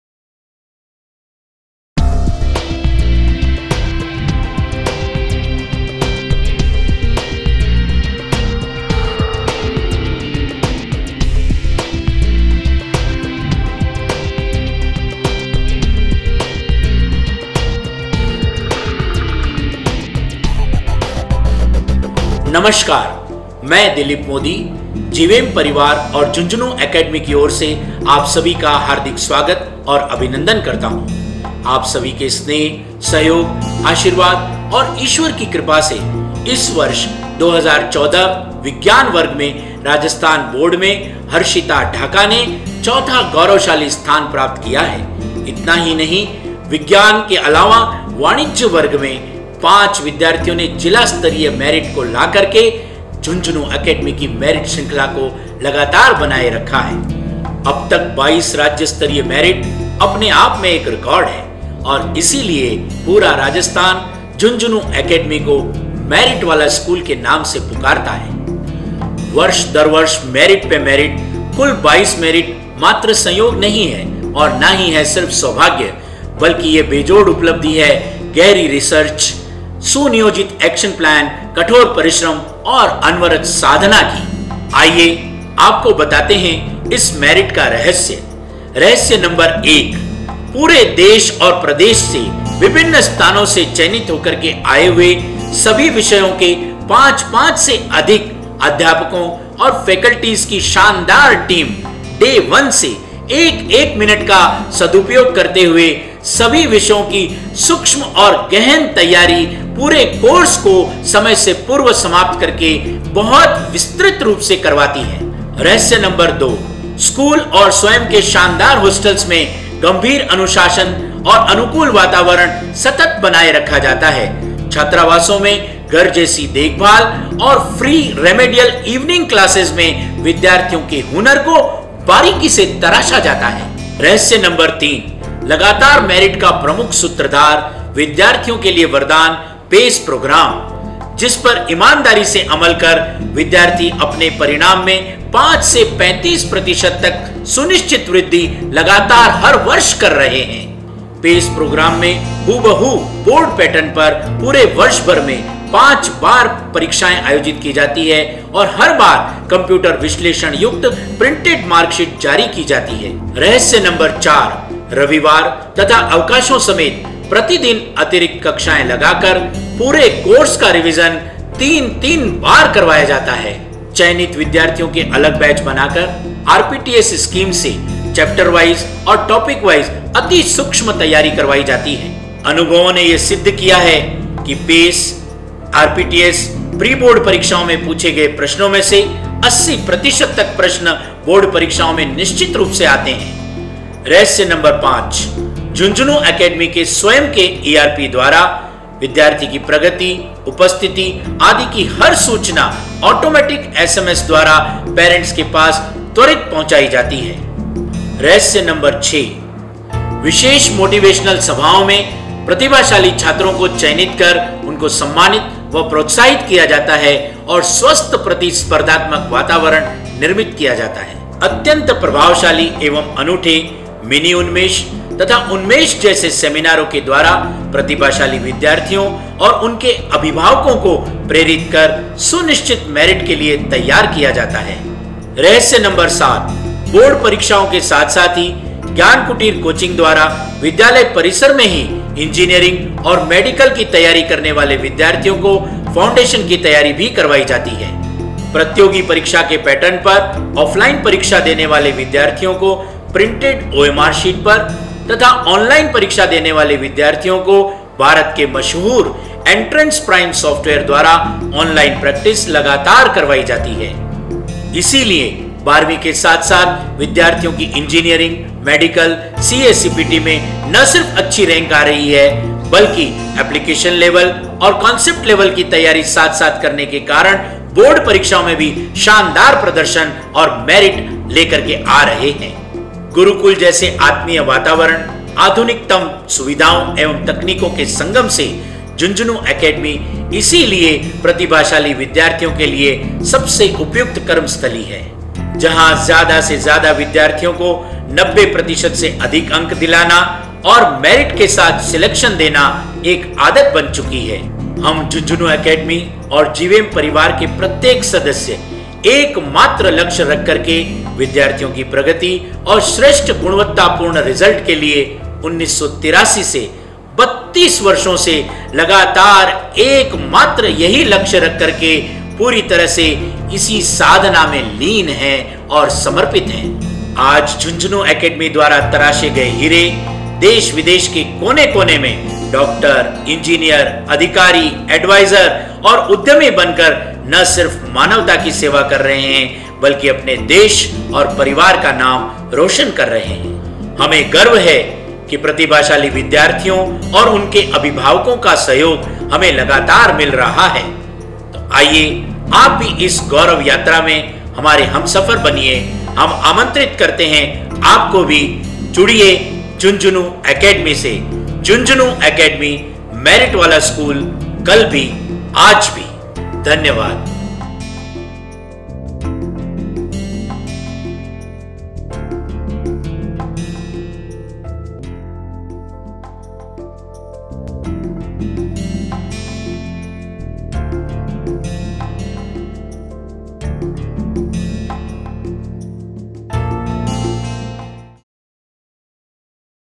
Merit ka Rahasya
Once again Jhunjhunu   Academy, the Merit Wala School, proved its metal by registering its presence at 4th Position in State Board Merit list in 2014 Science Result. Harshita Dhaka d/o Mrs & Mr Manju-Rakesh Dhaka score 95.80% topped whole Shekhawati Region. Not only this, but 5 students of Commerce batch secure their position in Distt. Board Merit List too.
Dr Dilip Modi, Chairman, JIVEM Education and of course, the inspiration behind these great achievement speaks on the Secret of Merit...